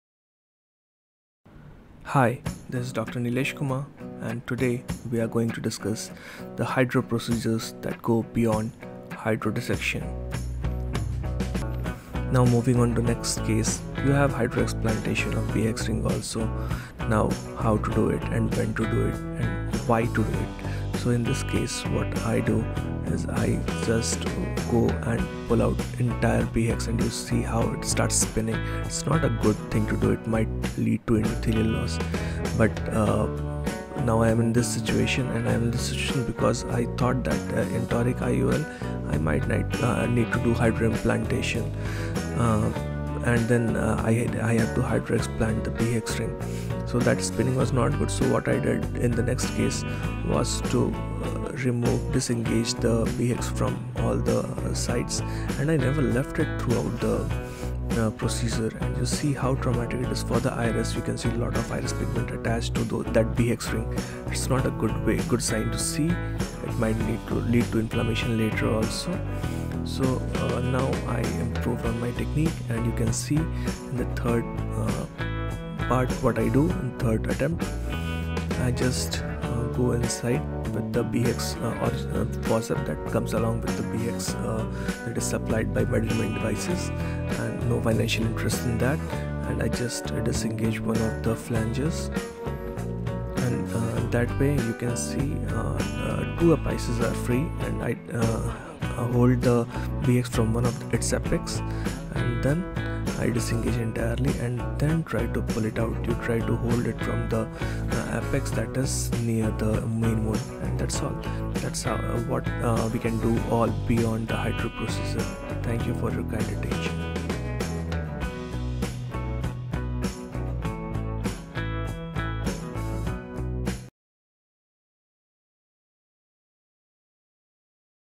B Hex HydroExplant Tips and Tricks
This is a part of the full-length 8-minute video submitted to the AIOC 2024 Video Competition which was selected in the top 50 videos for Podium Presentation.

Performed by Dr. Nilesh Kumar at madhavinetralaya